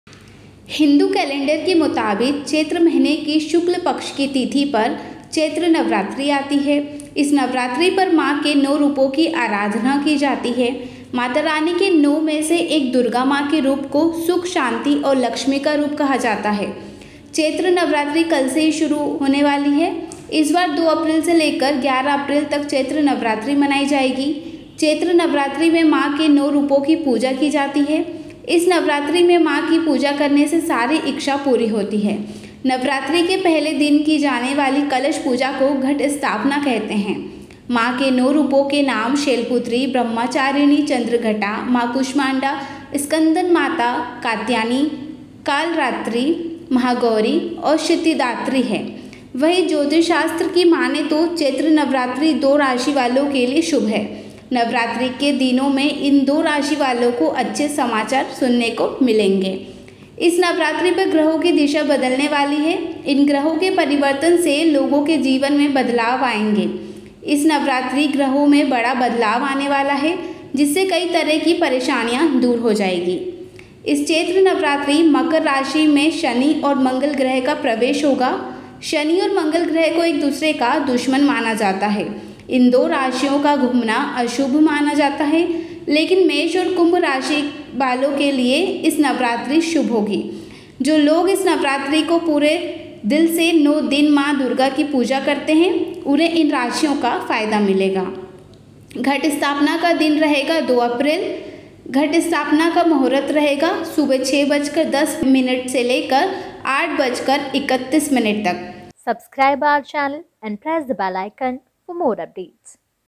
चैत्र नवरात्रि पर इन 2 राशियों को होगा बड़ा लाभ, बदल जाएगी किस्मत
इस नवरात्रि ग्रहों में बड़ा बदलाव आने वाला है,जिससे कई तरह की परेशानियां दूर हो जाएगी।
इस चैत्र नवरात्रि मकर राशि में शनि और मंगल ग्रह का प्रवेश होगा।

इस चैत्र नवरात्रि मकर राशि में शनि और मंगल ग्रह का प्रवेश होगा। शनि और मंगल ग्रह को एक दूसरे का दुश्मन माना जाता है। इन दो राशियों का घूमना अशुभ माना जाता है लेकिन मेष और कुंभ राशि के वालों के लिए इस नवरात्रि शुभ होगा। जो लोग इस नवरात्रि पुरे दिल से 9 दिन मां के रूपों की पूजा करेगा उन्हें इन राशियों का फायदा मिलेगा।

In this Chaitra Navratri Capricorn, Saturn and Mars will enter. Saturn and Mars are considered enemies of each other. The rotation of these two zodiac signs is considered inauspicious, but this Navratri will be auspicious for the people of Aries and Aquarius. Those who worship the forms of Mother with all their heart for 9 days this Navratri will get the benefit of these zodiac signs.